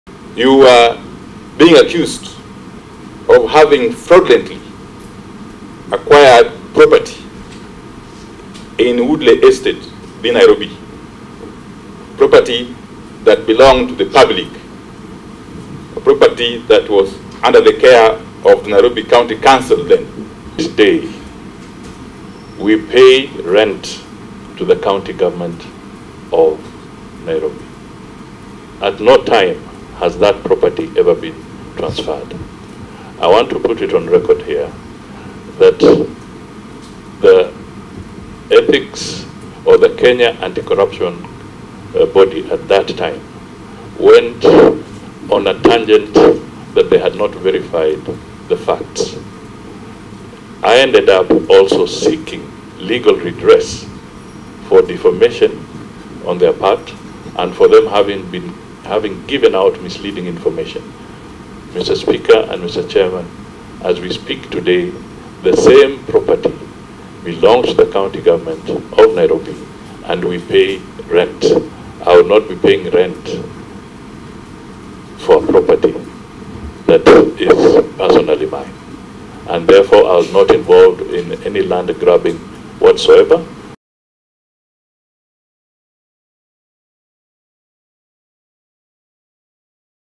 Mudavadi: I didn't grab Woodley property, it belongs to Nairobi
I didn't grab the Woodley estate property, Prime Cabinet Secretary designate Musalia Mudavadi has clarified.